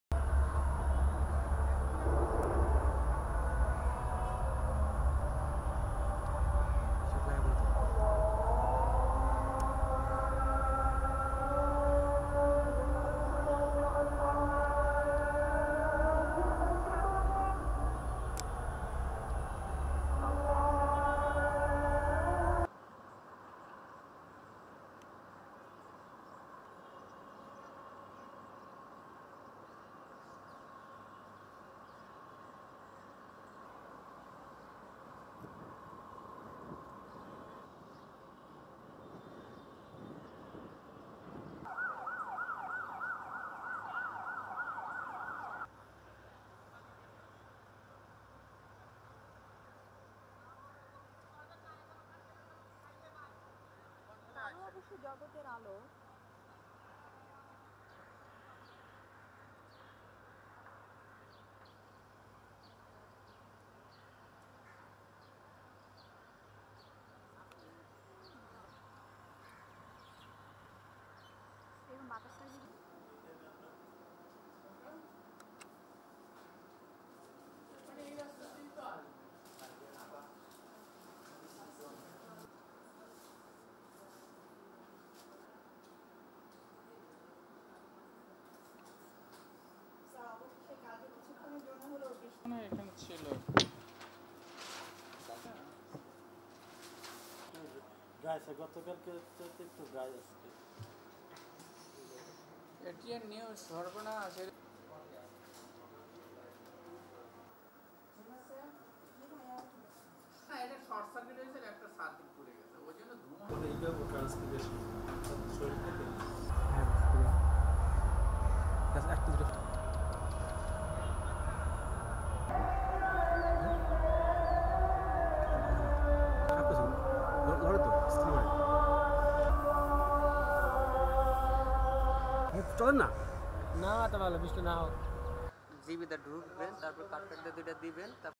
General View of Bangladesh Meteorological Department Dhaka During Rainy Day
news & current affairs